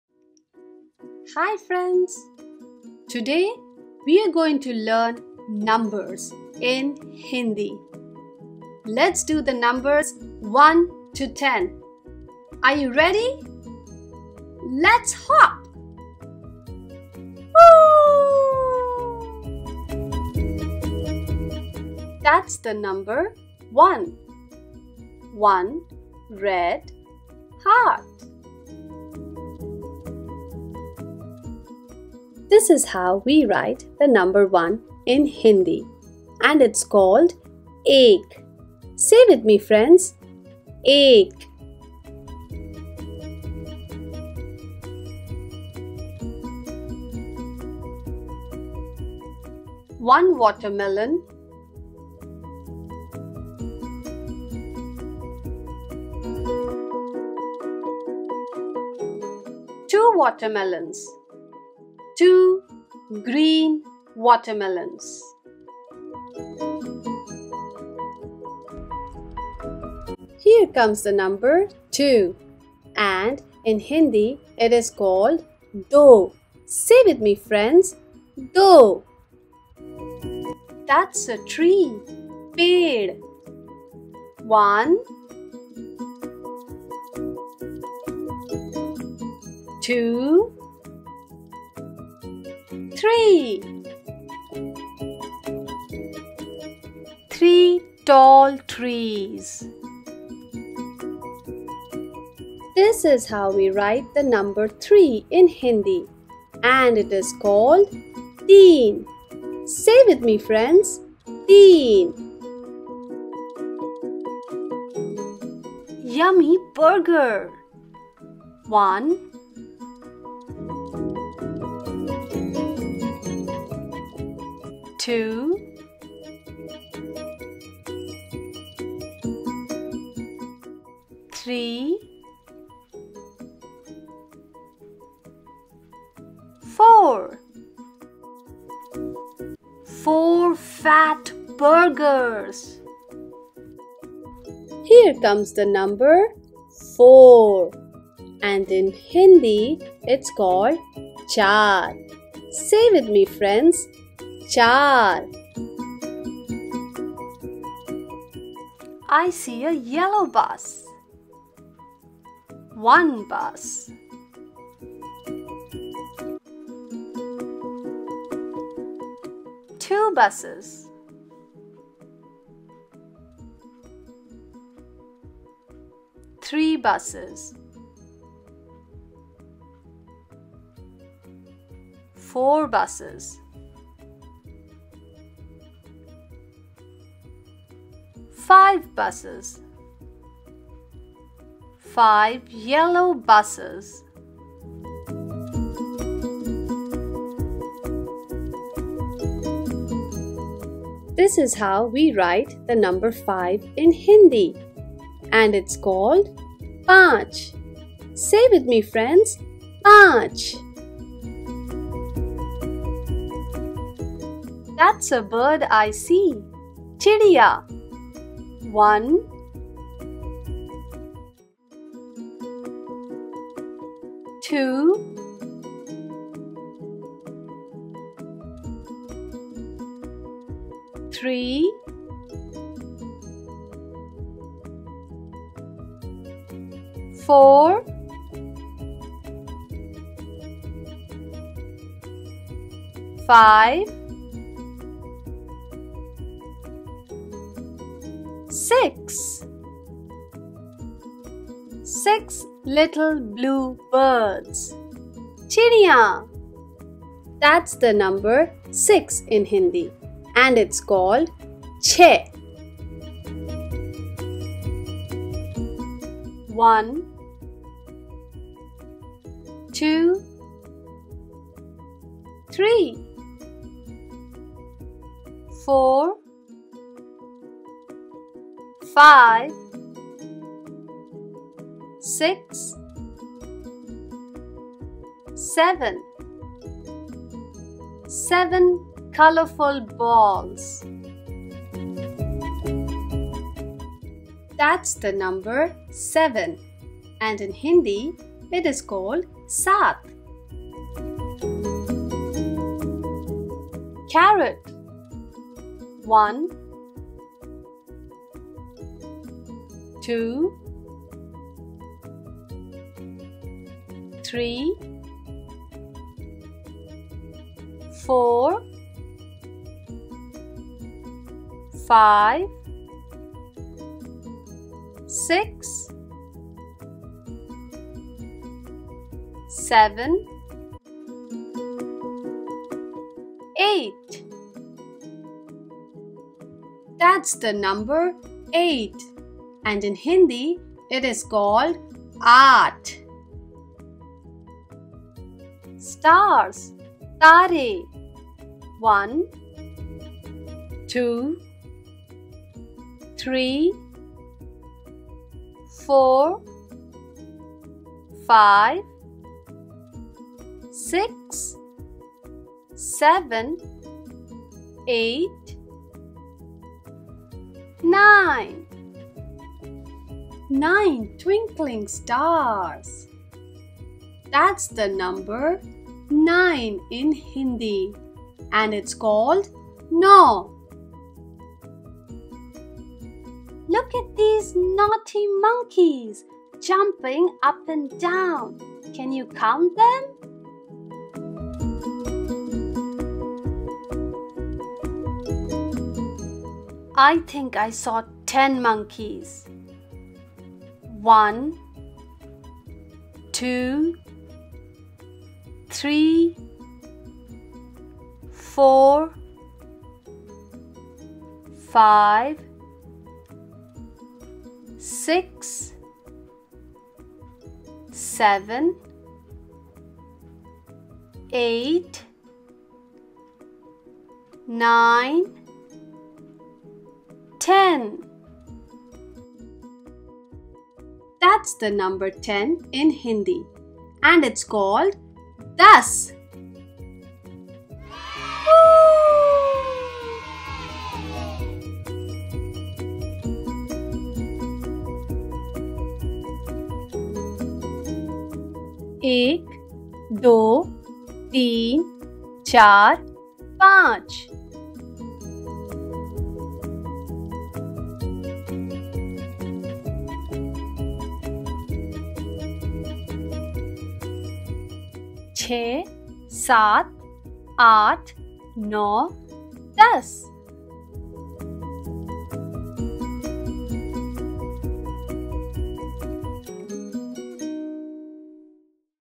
Learn 1 to 10 | English and Hindi Numbers
Learn 1 to 10   English and Hindi Numbers. In this bilingual video, kids can learn Hindi numbers from 1 to 10.